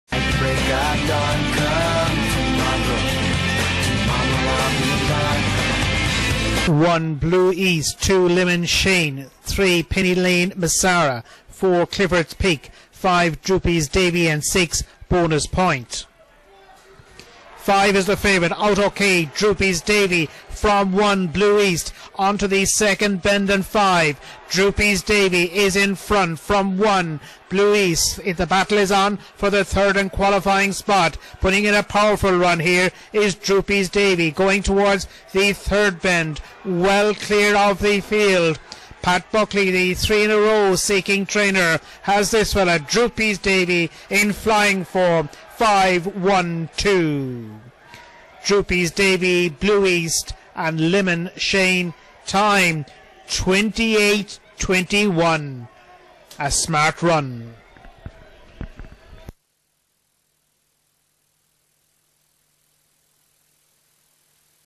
IGB - 2018 Con & Annie Kirby Memorial Stake Semi-Final 14/04/2018 Race 11 - Limerick
Stadium: 2018 Con & Annie Kirby Memorial Stake Semi-Final
Track Name: Limerick
Date: 14/04/2018
Hour: 22:09

Results

1st    5     DROOPYS DAVY        1/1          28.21
2nd    1     BLUE EAST           5/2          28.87
3rd    2     LEMON SHANE         5/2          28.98
4th    6     BONUS POINT         8/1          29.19
5th    4     CLIFFORDS PICK      12/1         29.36
6th    3     PENNYLANE MASARA    6/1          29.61

|LMK_2018_04_14_11